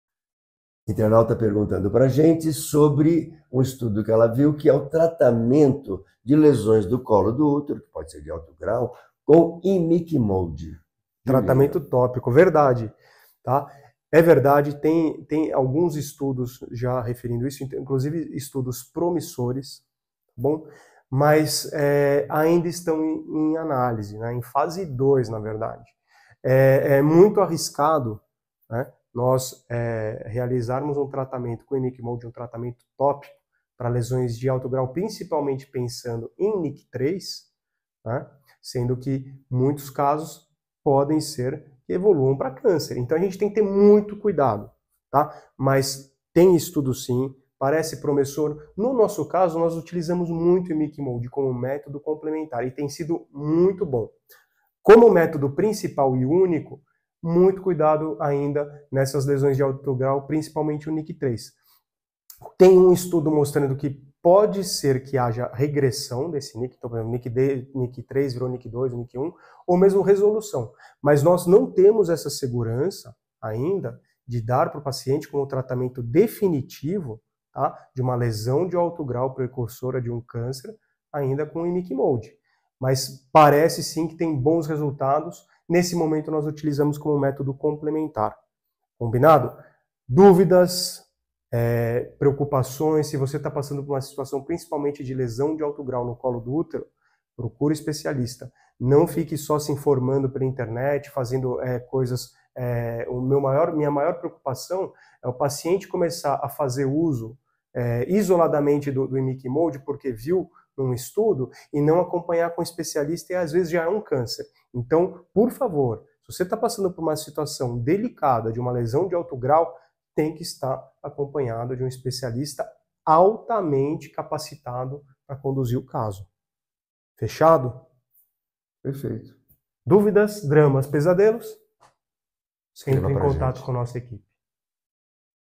Tratamento de lesões de alto grau no colo do útero com imiquimode é possível?
Pergunta de uma seguidora nossa,
Aliás pergunta extremamente interessante e atual!